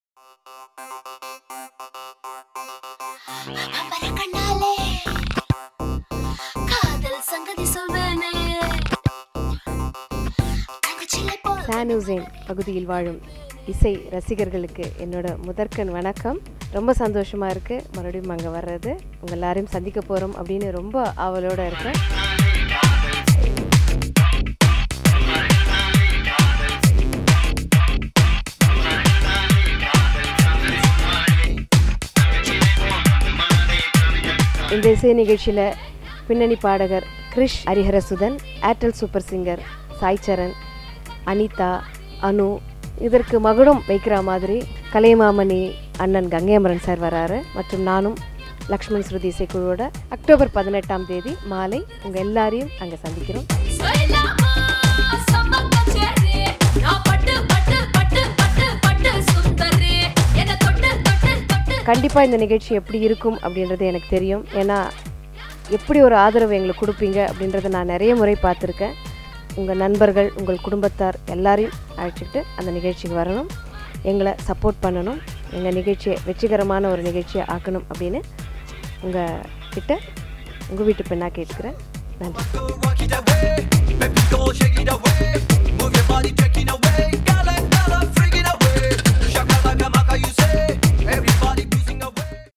Lakshman Sruthi Orchestra San Jose Singer Malathy Promo Video
Kalalaya USA & Lakshman Sruthi Present USA & Canada Music Journey 2014
San Jose 18th Oct 2014 @ 6pm, Chabot College Auditorium, Hayward, CA.